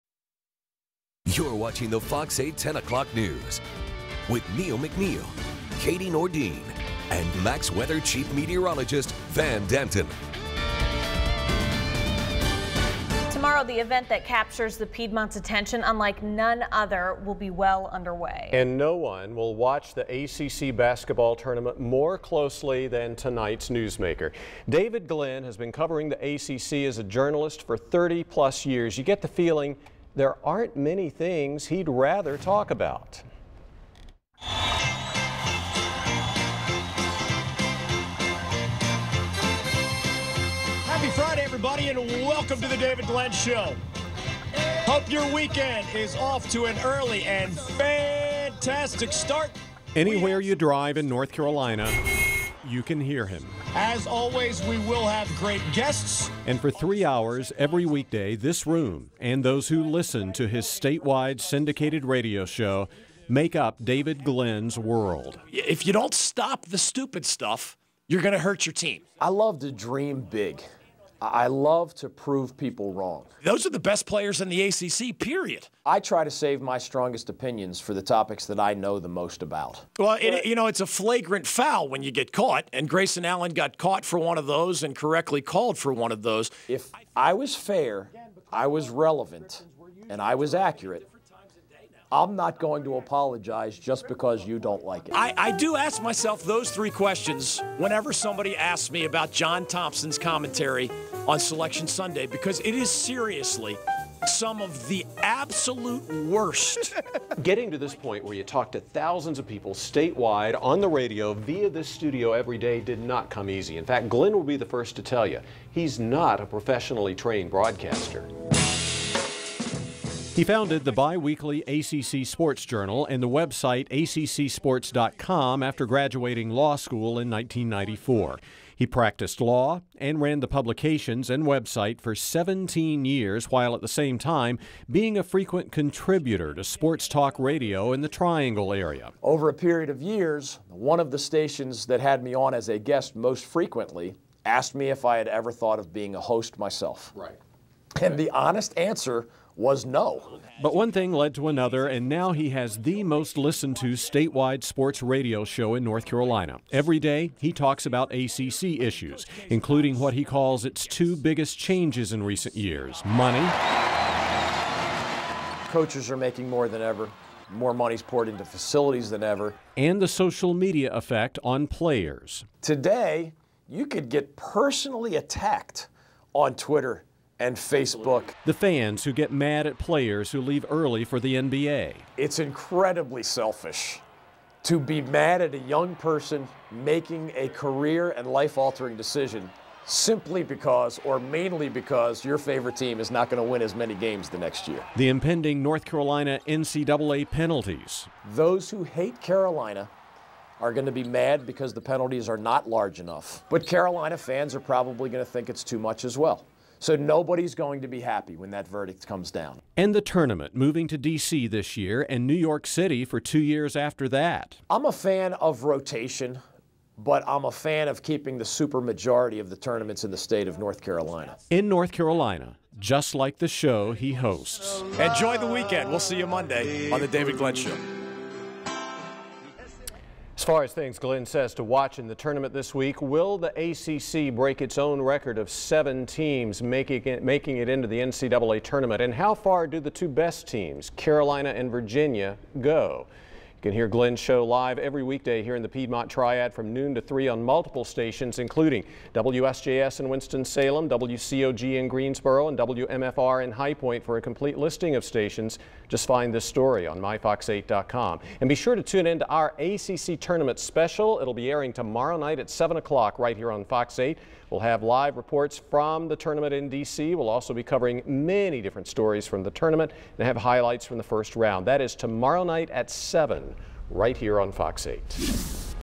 WGHP NEWSMAKER: DAVID GLENN
Neill McNeill's Newsmaker feature on NC syndicated sports talk radio host David Glenn of The David Glenn show as aired in 10pm News 3/7/16.
WGHP-TV FOX8
Greensboro/High Point/Winston-Salem, North Carolina